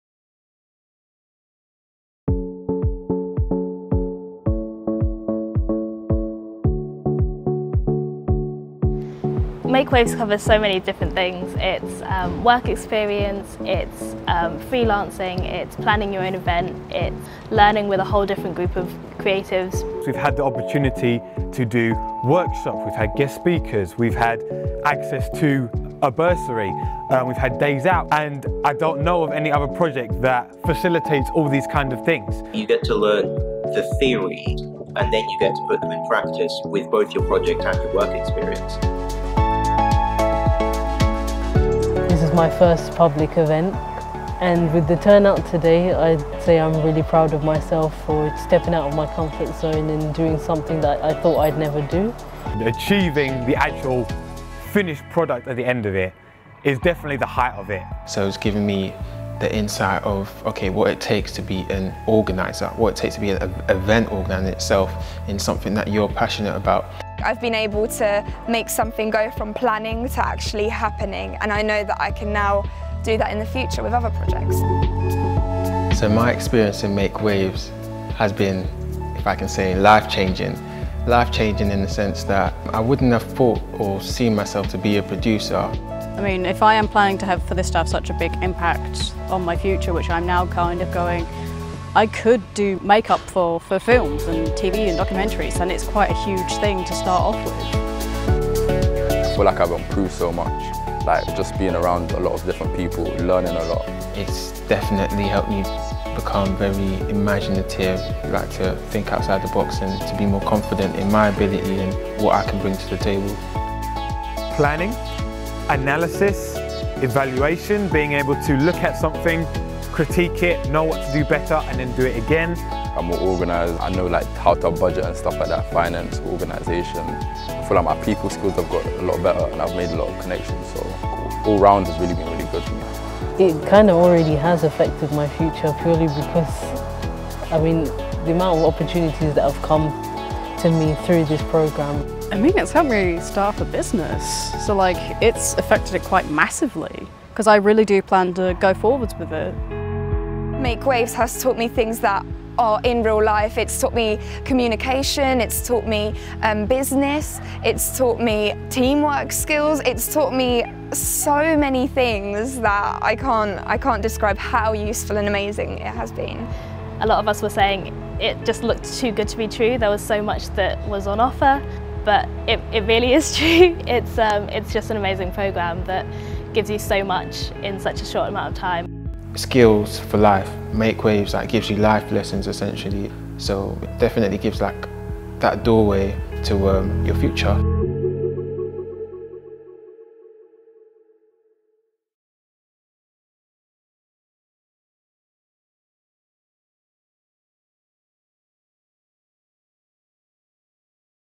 Make Waves (2022)
We are looking for the next generation of event makers and creators in Medway.
Make Waves is the way for 18-25 year olds who live, work or study in Medway to get their foot in the door of the creative industries and start out on a career path like no other.